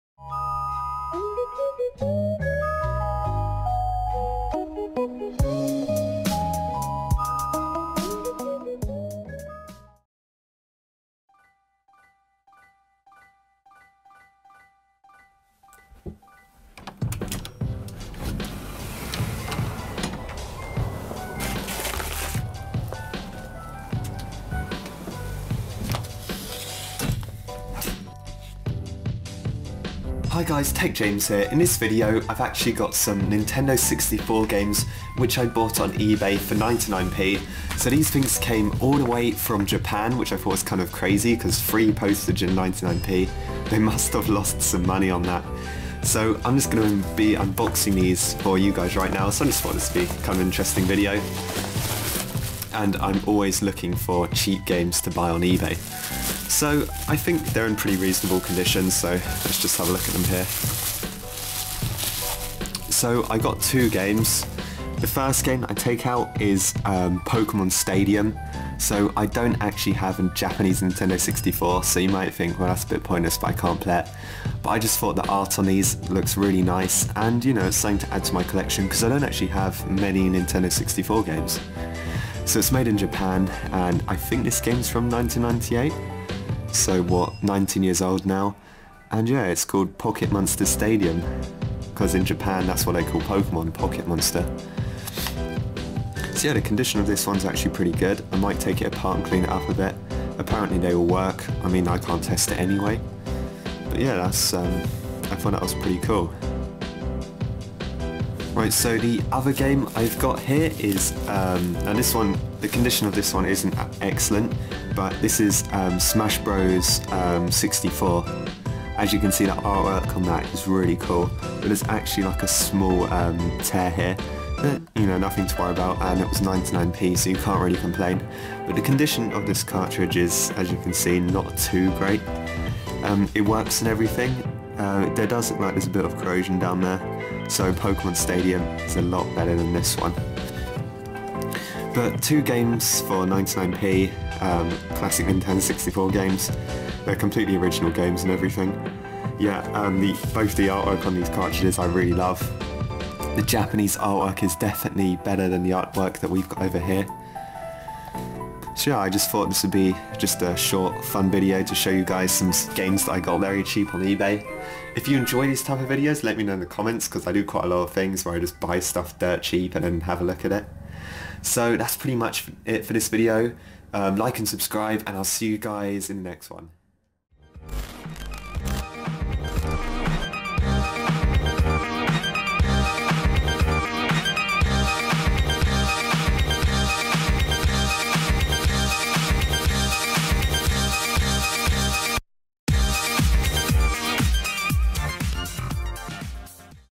$0.99 Japanese Nintendo 64 Games Unboxing! (eBay Finds)
In this video, I’ll be unboxing 2 Japanese Nintendo 64 Game that I got off eBay for $0.99!